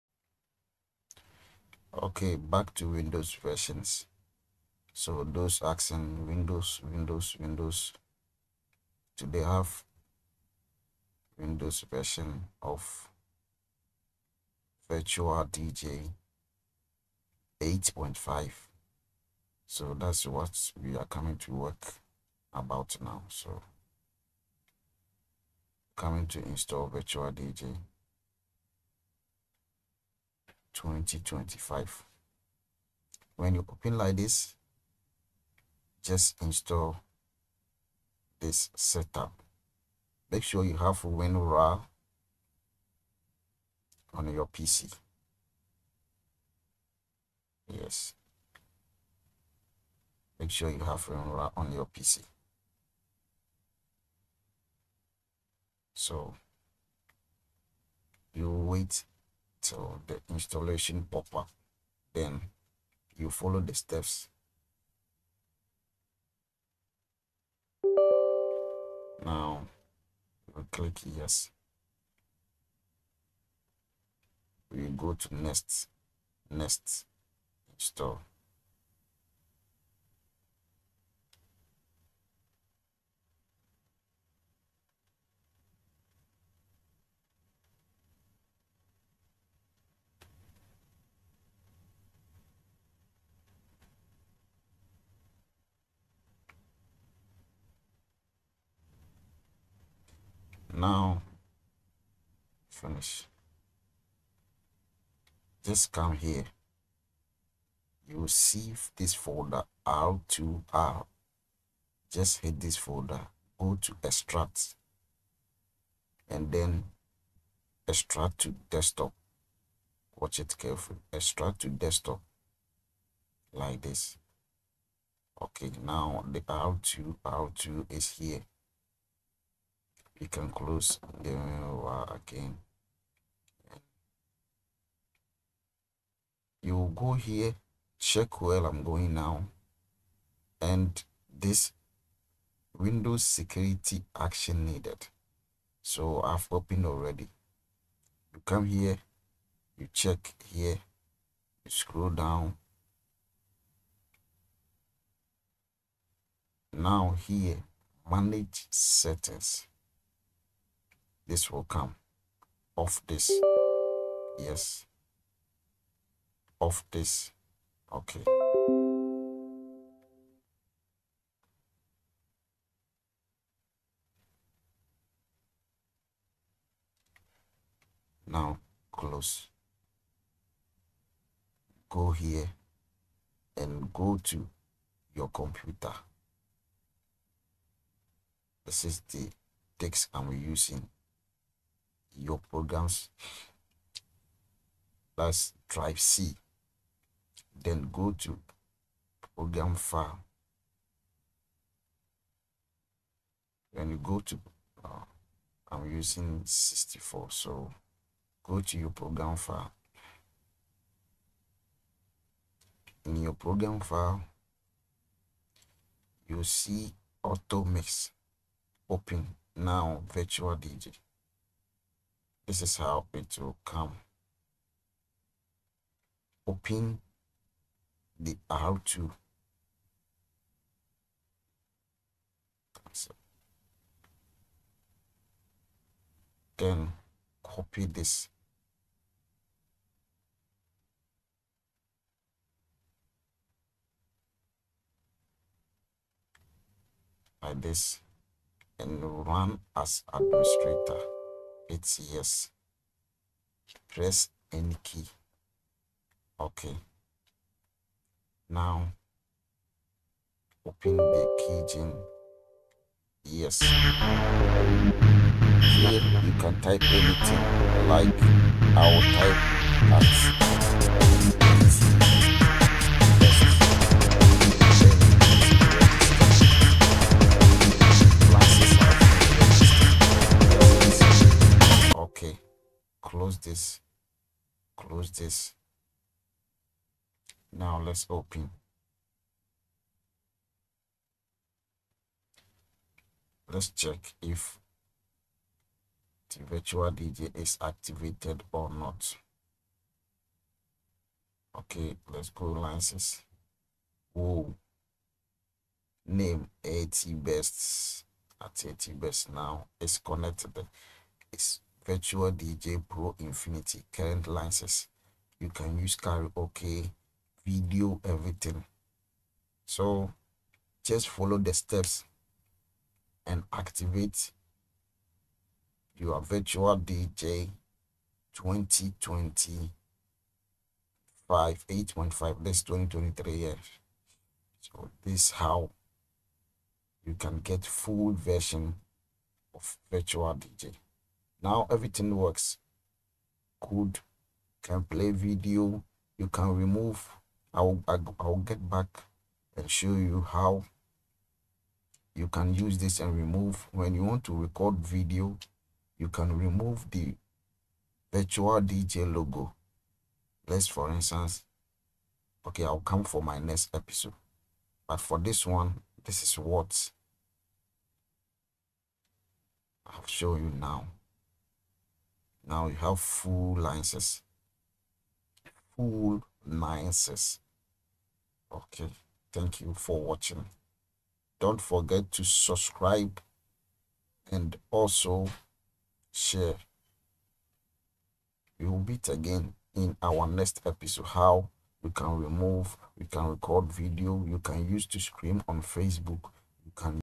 HOW TO INSTALL VIRTUAL DJ 2025 PRO INFINITY ON WINDOWS LAPTOP
How to install the full version of virtual dj pro infinity.